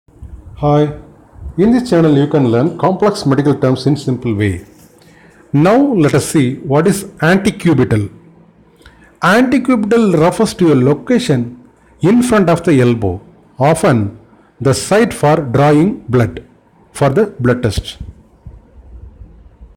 What is antecubital?
Antecubital is explained in this video.

In This channel you can learn complex medical terminology in simple way..